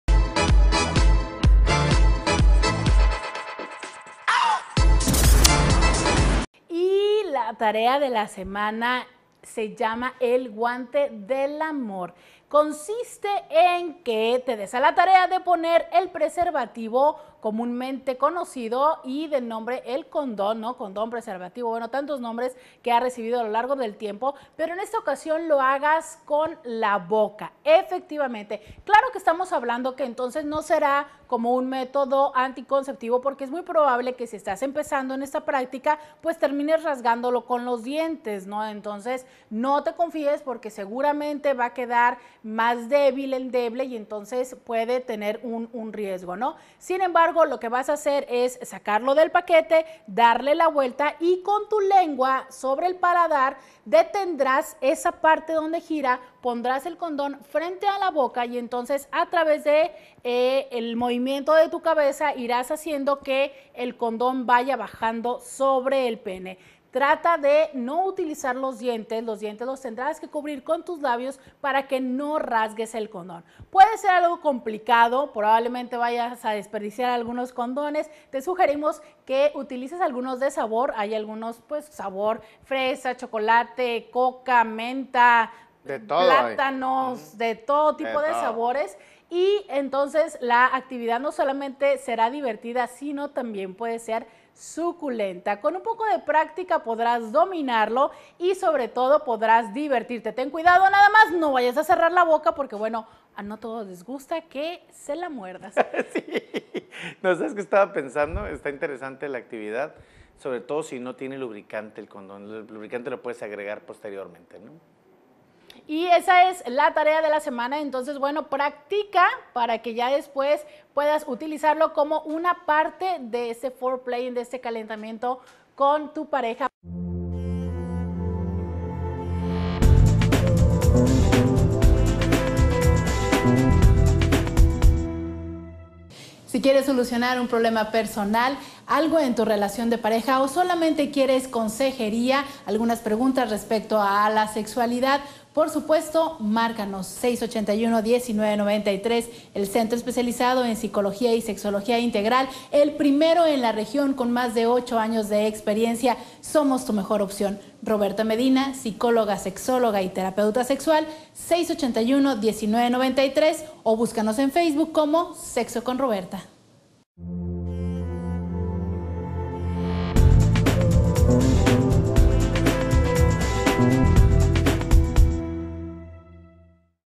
Sexo con Robertha: Tarea Sexual de la Semana
Robertha Medina Psicóloga y Sexóloga hablando de: la tarea sexual de la semana.